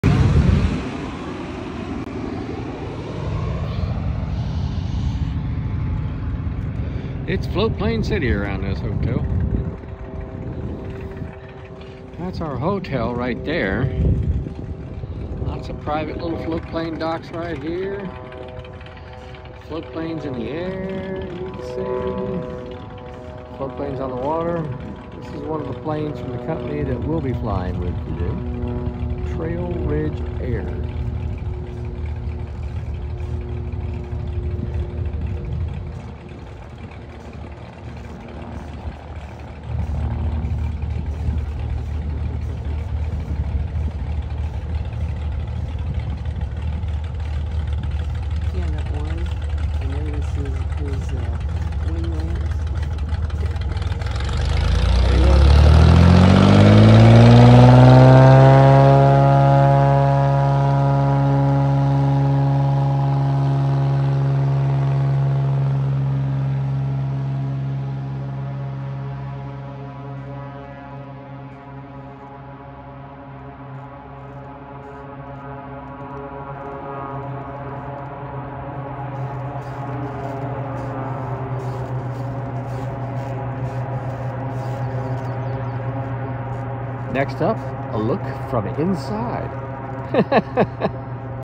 Trail Ridge Air - Anchorage Alaska - Seaplane Takeoff - 05/20204 (From the Outside of the plane)
Trail Ridge Air - Anchorage Alaska - Seaplane Takeoff - 05/20204